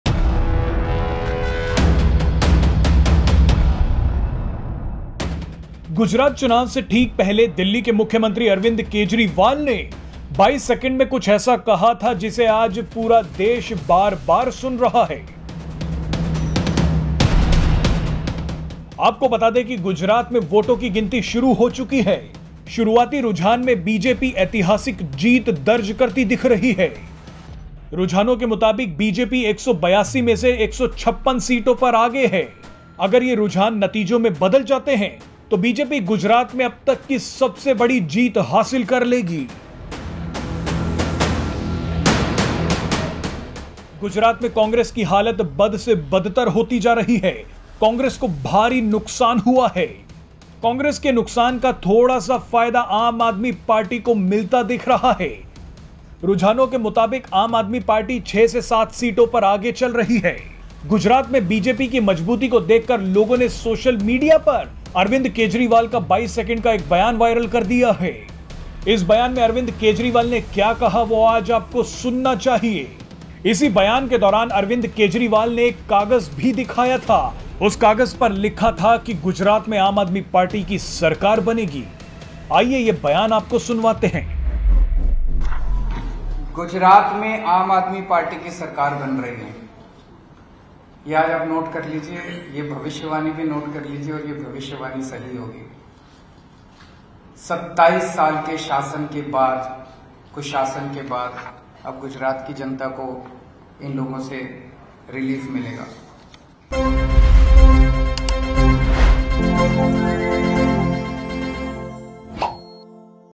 कागज पर ये क्या लिख कर लाए Kejriwal, हंस रहे हैं Modi | Gujarat,Assembly Election Results 2022 Live
Delhi Chief Minister Arvind Kejriwal said something in 22 seconds just before the Gujarat elections, which the whole country is hearing again and again today. Let us tell you that in Gujarat BJP is seen to register a historic victory.

गुजरात चुनाव से ठीक पहले दिल्ली के मुख्यमंत्री अरविन्द केजरीवाल ने 22 सेकंड में कुछ ऐसा कहा था जिसे आज पूरा देश बार बार सुन रहा है. आपको बतां दे कि गुजरात में बीजेपी ऐतिहासिक जीत दर्ज करती दिखाई दे रही है.